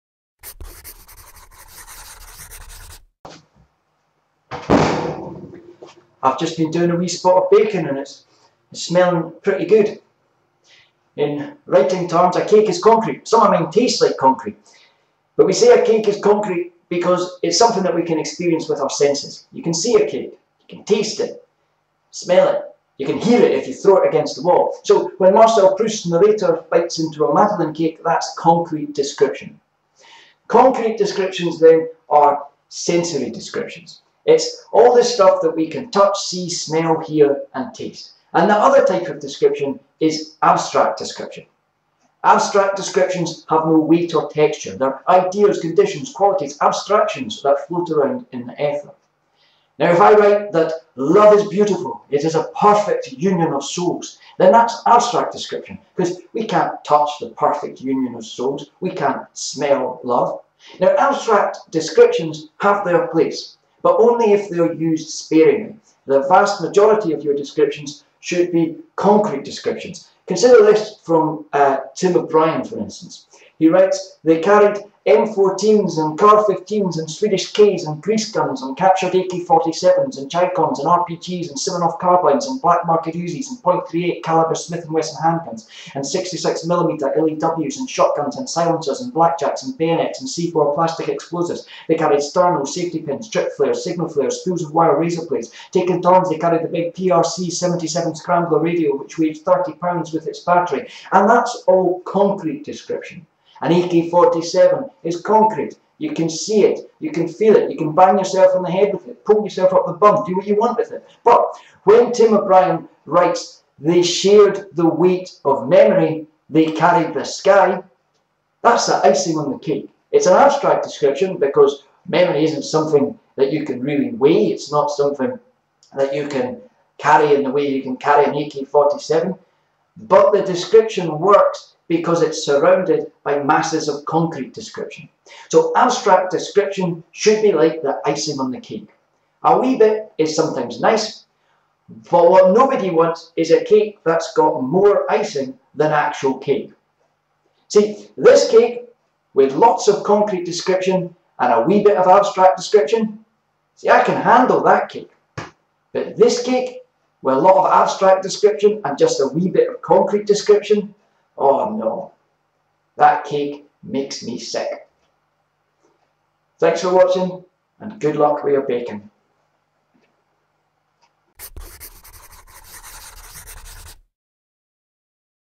Concrete versus abstract description (writing tip 30)
Concrete description is sensory description; it is the stuff we can touch, see, smell, hear, and taste. Abstract descriptions have no weight or texture.  They are ideas, conditions, qualities, abstractions that float around in the ether. When and how should writers use these different types of description?

Video transcript follows below:

I’ve been doing a wee spot of baking – smells delicious. In writing terms, a cake is concrete – some of mine taste like concrete. We say a cake is concrete because it’s something that can be experienced by the senses. We can touch a cake, smell it, see it. So when Marcel Proust’s narrator eats a madeleine, that’s an example of concrete detail.

So concrete description is sensory description; it is the stuff we can touch, see, smell, hear, and taste. The other type of description is abstract description. Abstract descriptions have no weight or texture.  They are ideas, conditions, qualities, abstractions that float around in the ether.

If I write that ‘Love is beautiful. It is a perfect union of souls’ – that’s abstract description. We can’t touch the perfect union of souls or smell love.

Abstract descriptions have their place, but one of the most common writing problems I encounter is that inexperienced authors use too much abstraction. The vast majority of your description should be concrete. Consider this from Tim O’Brien:

They carried M-14s and CAR-15s and Swedish Ks and grease guns and captured AK-47s and Chi-Coms and RPGs and Simonov carbines and black market Uzis and .38 caliber Smith & Wesson handguns and 66 mm LAWs and shotguns and silencers and blackjacks and bayonets and C-4 plastic explosives. (...)  They carried Sterno, safety pins, trip flares, signal flares, spools of wire, razor blades (...).  Taking turns they carried the big PRC-77 scrambler radio, which weighed thirty pounds with its battery.

That’s all concrete description. You can get an AK47 and sniff it and see it and poke it up your bum. When O’Brien adds ‘They shared the weight of memory. They carried the sky.’ It’s the icing on the cake. It’s an abstract description – memory isn’t something with weight like an AK47 is – but it works because it’s surrounded by masses of concrete description.

So abstract description should be like the icing on the cake. A little bit is sometimes nice. What nobody wants is a cake that’s got more icing than actual cake. This cake, with lots of concrete description and a little bit of abstract description? I can handle that. But this cake, with a lot of abstract description and just a wee bit of concrete description? That cake makes me sick.